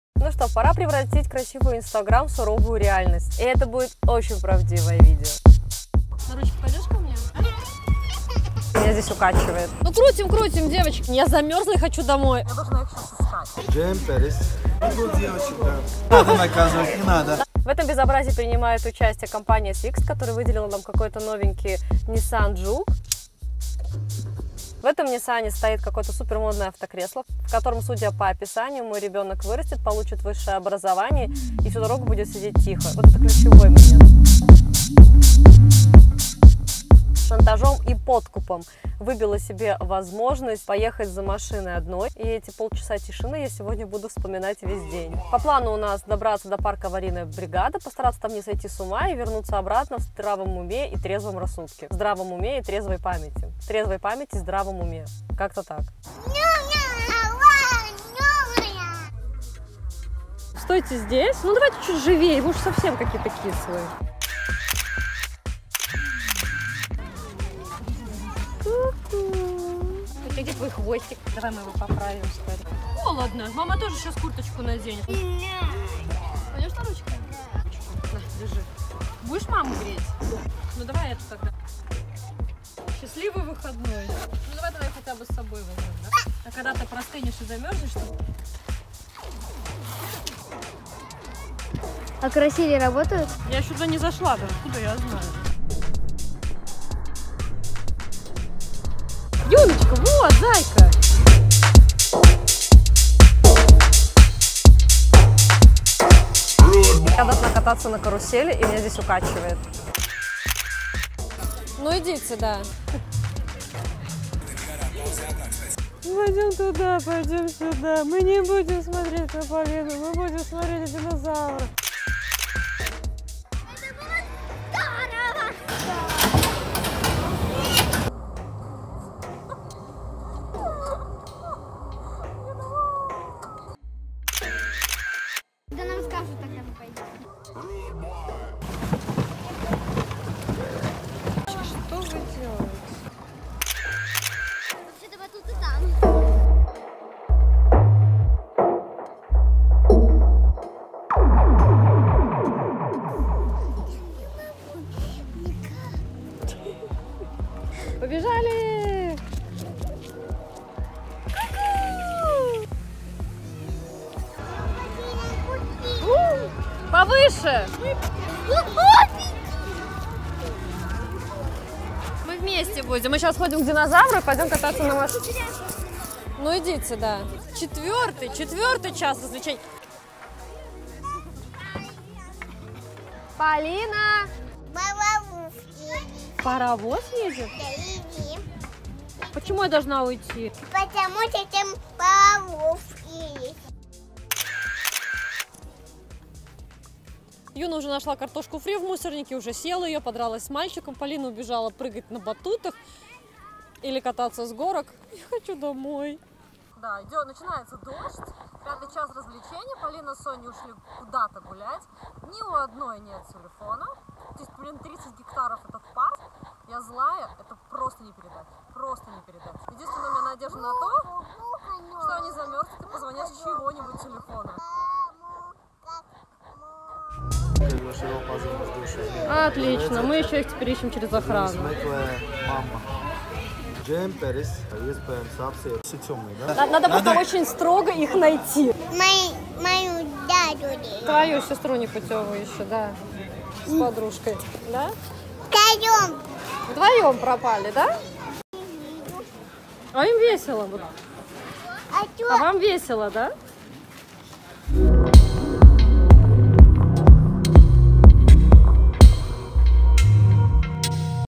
ОБОЗРЕЛА: Один день в парке Avarījas Brigāde: красивая картинка vs реальность
Что скрывается за красивыми картинками - вот взяла и честно рассказала. Но красивые картинки сделала. Но и рассказала честно